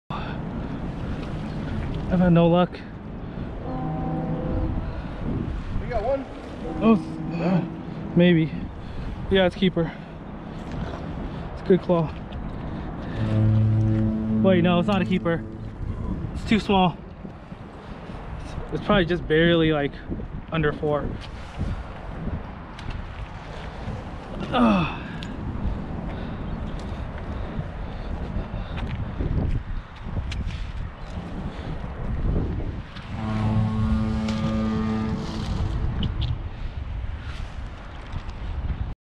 Using a small crab pot for Rock Crabs in San Francisco
Using a small crab pot on the pier for Rock Crabs in San Francisco, CA.